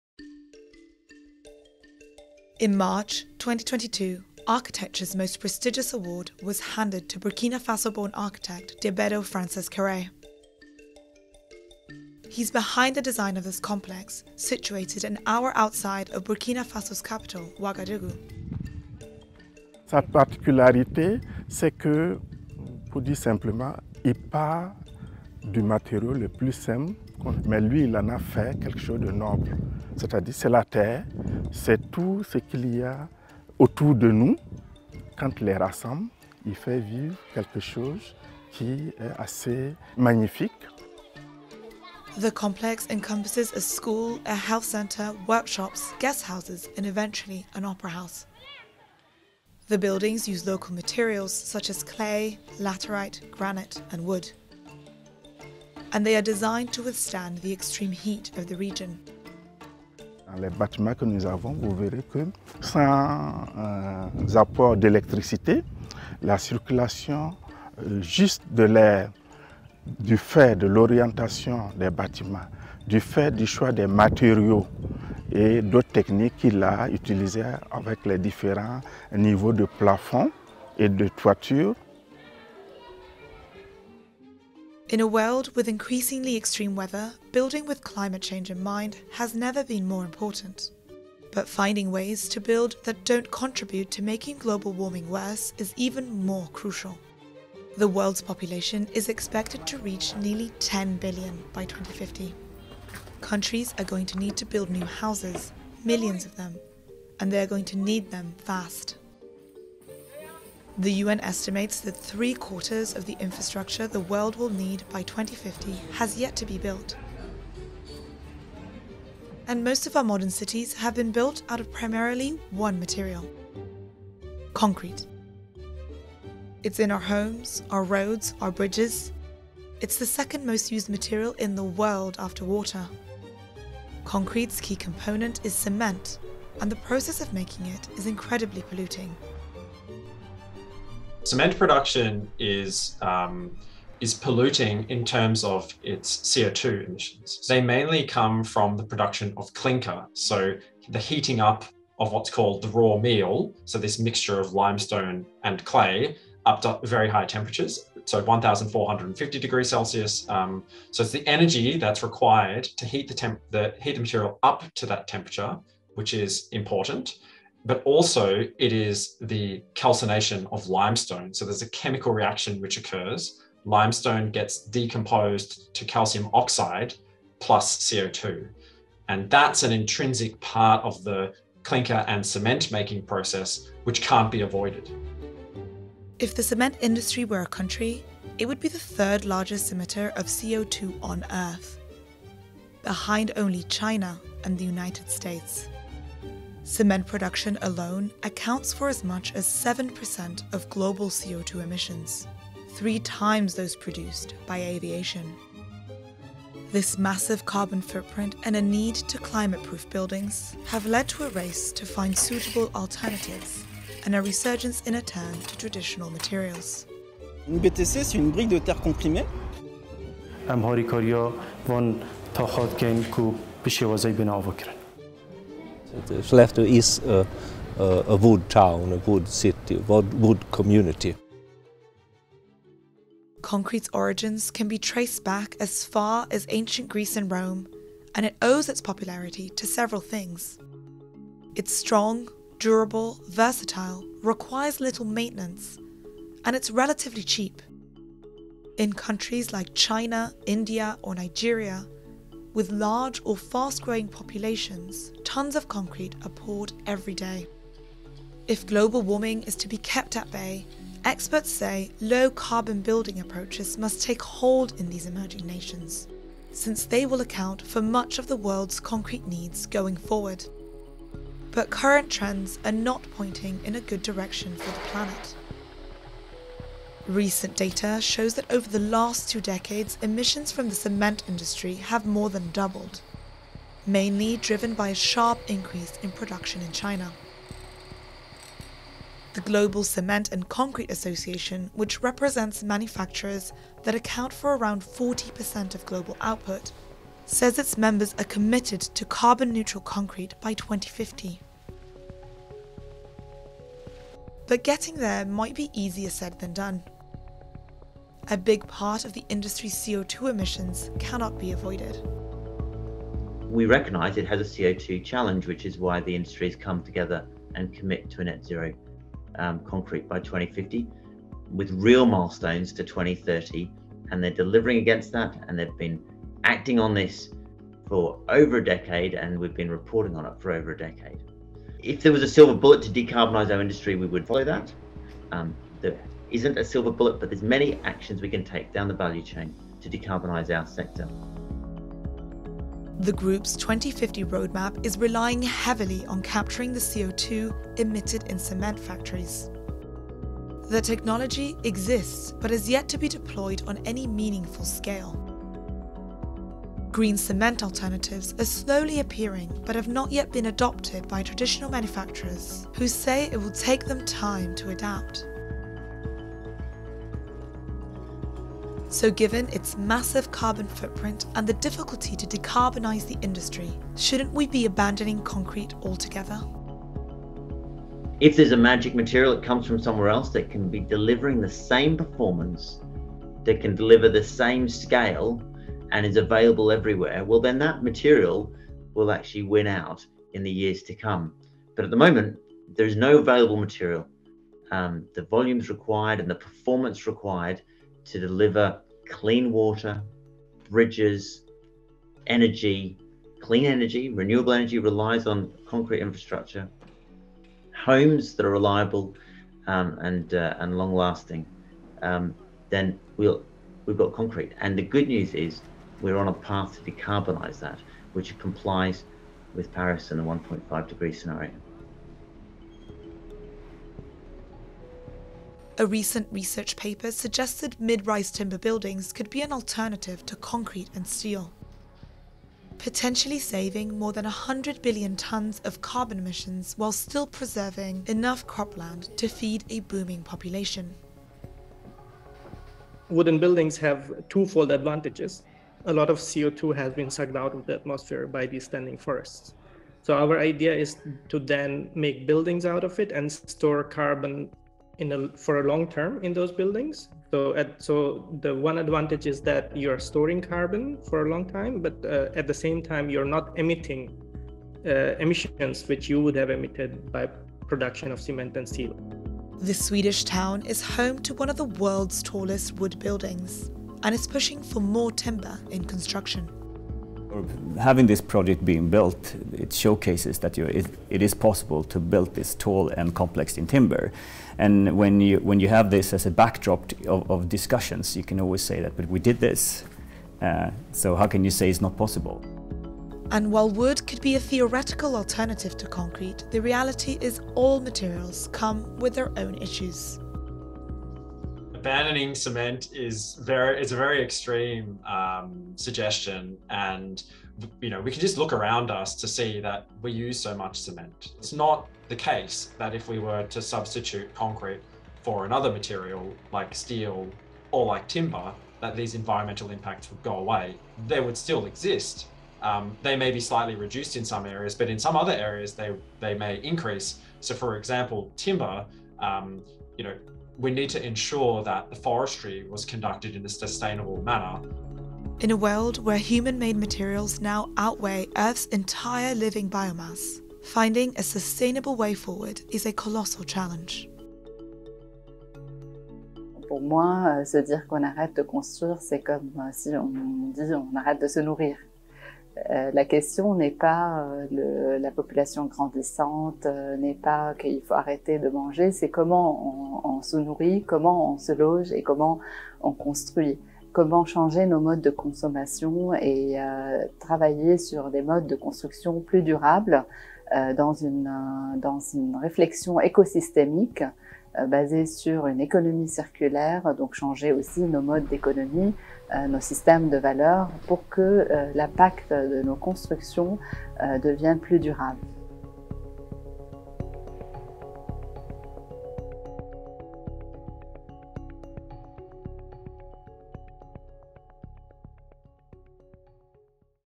Cement is the world’s 3rd largest CO2 emitter, can it shed its massive carbon footprint? | AFP
If the cement industry were a country, it would be the third largest emitter of CO2 on Earth, behind only China and the United States. Cement is used to make concrete, an essential material in global housing, construction and infrastructure, but can it be made less damaging to the planet? As countries come together at the COP27 summit in Egypt's Sharm el-Sheikh, the UN’s chief has warned that nations must cooperate or face "collective suicide" in the fight against climate change, at a summit where developing countries reeling from global warming demanded more action from rich polluters. "Humanity has a choice: cooperate or perish," Antonio Guterres said.